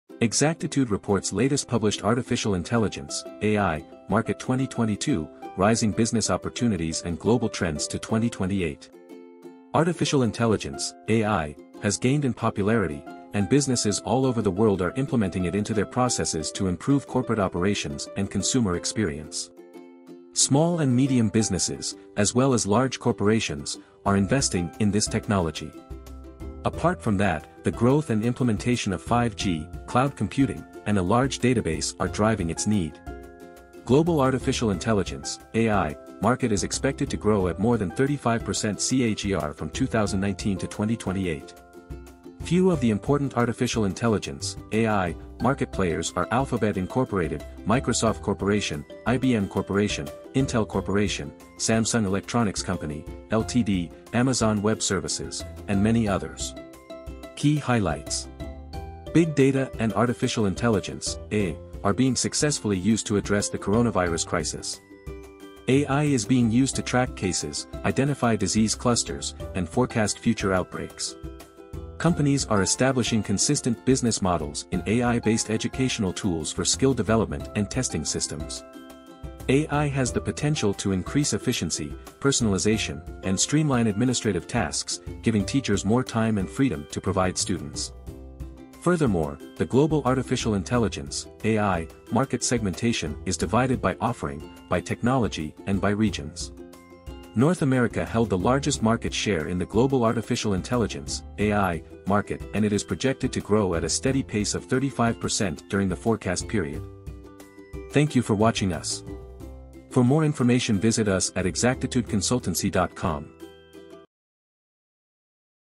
Artificial Intelligence (AI) Market | Exactitude Consultancy Reports
ExactitudeConsultancy Latest Published ArtificialIntelligence (AI) Market Research Report 2022 by Offering (Hardware, Software, Services), Technology (Machine Learning, Natural Language Processing), Business Function (Law, Security), Vertical, and Region (North America, Europe, Asia Pacific, South America, Middle East and Africa), Global trends and forecast from 2023 to 2028

The global artificial Intelligence (AI) market is expected to grow at 35% CAGR from 2023 to 2028. It is expected to reach above USD 521.3 billion by 2028 from 35 USD billion in 2022.

Artificial intelligence market is segmented on the basis of offering, technology, deployment mode, organization size, business function, vertical, and region

Based on offering, software led the artificial intelligence market, accounting for more than 36% of global revenue. This high share can be attributed to prudent improvements in data storage capacity, computing power, and parallel processing capabilities to deliver high-end AI software in dynamic end-use verticals. Libraries for designing and deploying artificial intelligence applications, such as those for primitives, linear algebra, inference, sparse matrices, video analytics, and multiple hardware communication capabilities, are included in artificial intelligence software solutions. The need for enterprises to understand and analyse visual content in order to gain meaningful insights is expected to drive the adoption of artificial intelligence software.

Based on technology, deep learning dominates the global market. This high share can be attributed to its popularity as a result of its complex data-driven applications, such as text/content or speech recognition. Deep learning provides lucrative investment opportunities by assisting in the resolution of high data volume challenges. For example, in July 2020, Zebra Medical Vision partnered with TELUS Ventures to expand AI solutions to clinical care settings and new modalities and increase the availability of the former’s deep learning solutions in North America.